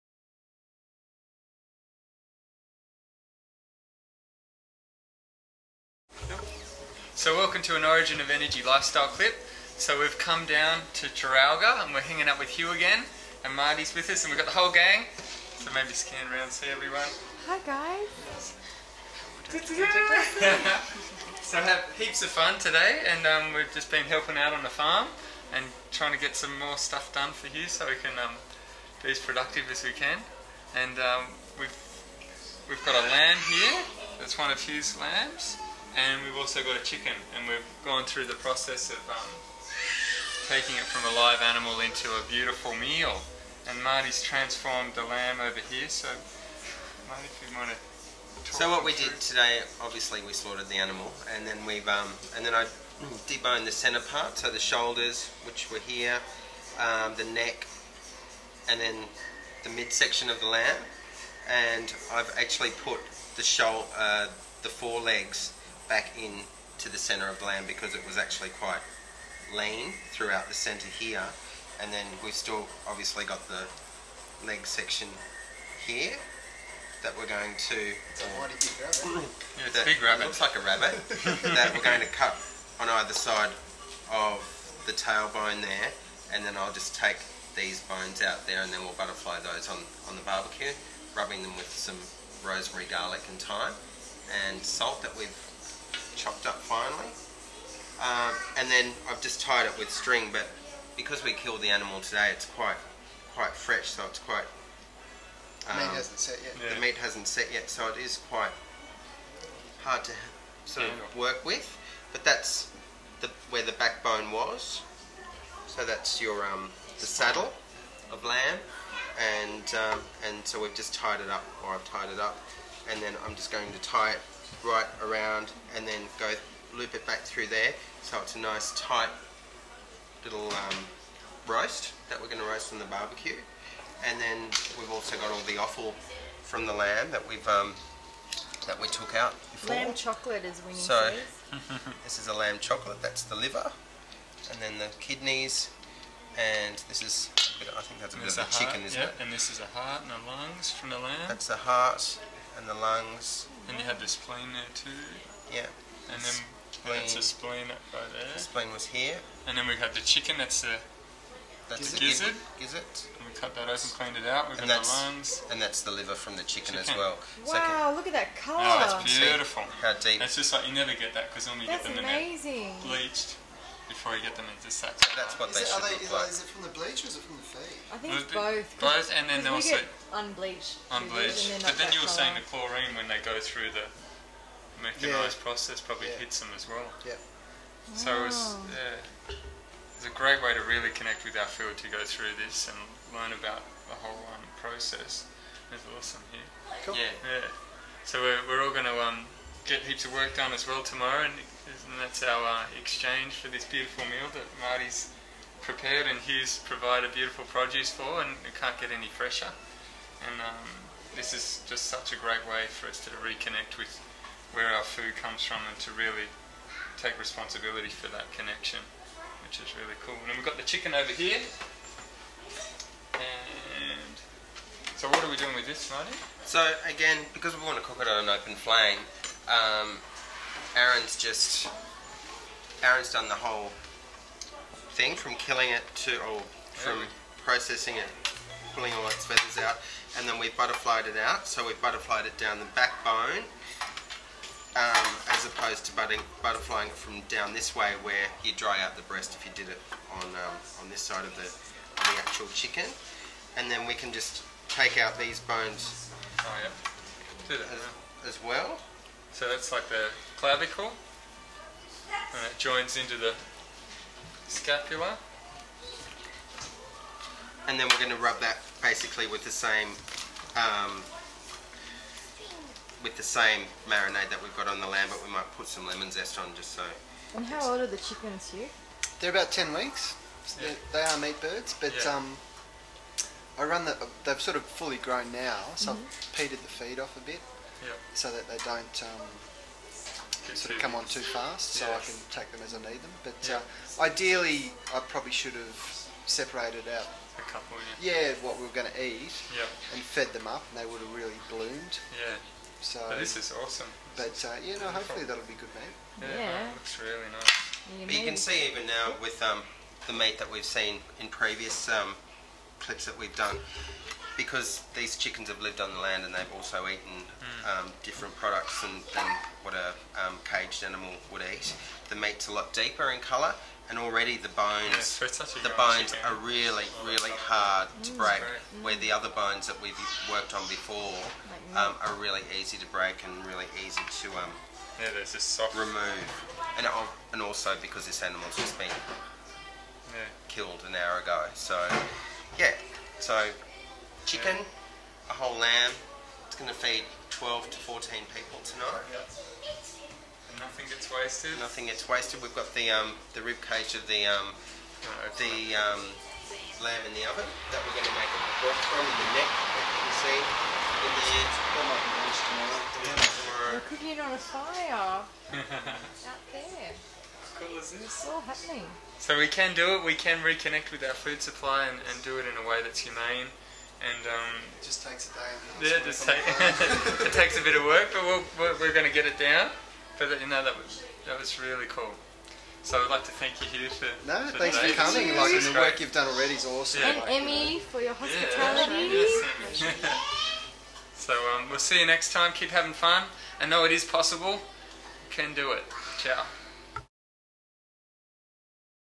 Hughs Farm 041008
Trip to Taralga to visit Hugh, Mary Ellen and their kids Henry, Adelaide & Winnie on their farm. We had an awesome farming experience and learnt how process a lamb and chicken then turn them into an awesome meal. Keep having fun, together we can do this, freedom for all through taking responsibility for ourselves:)